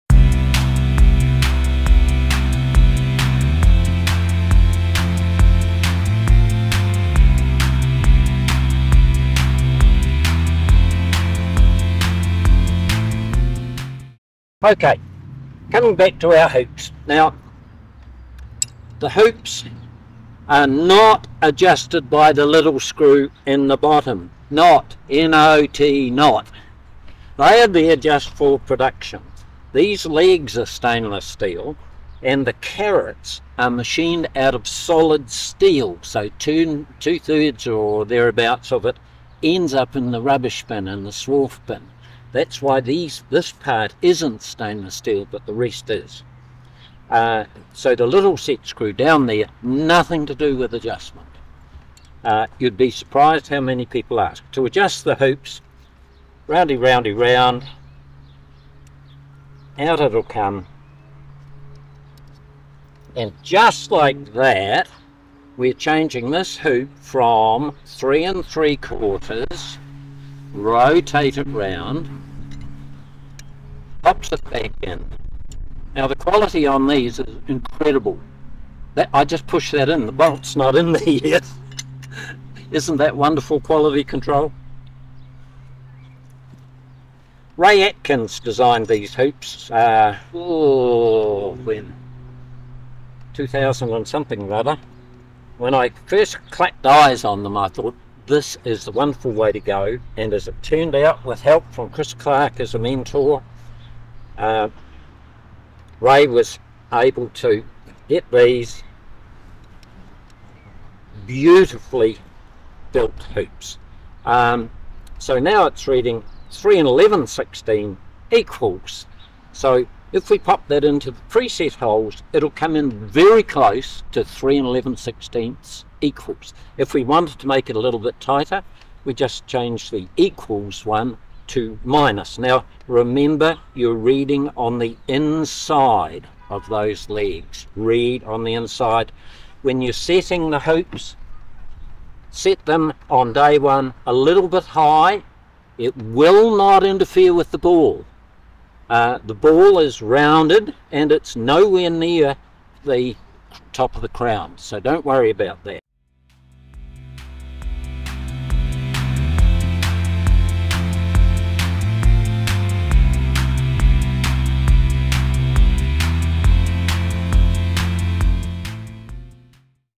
4 Quadway Hoop Adjustment
This is the forth video in a series produced with Mike McClure from Terminator Mallets and Quadway Hoops designed to help you set up your Croquet Courts.